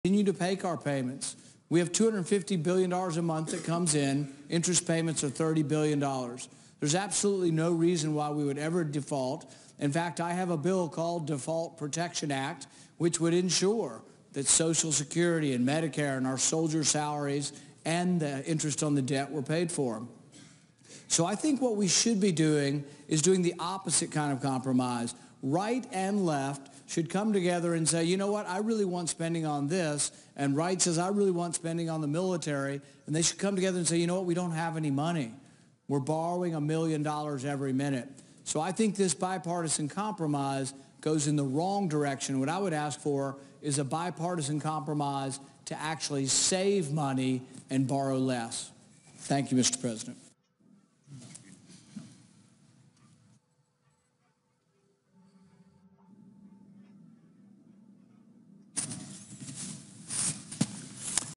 Rand Paul on Senate floor 10-29-2015 from CPSAN
LetRandSpeak "I rise today in opposition to the debt ceiling without getting any sort of spending reform, or budgetary reform in exchange"
CSPAN2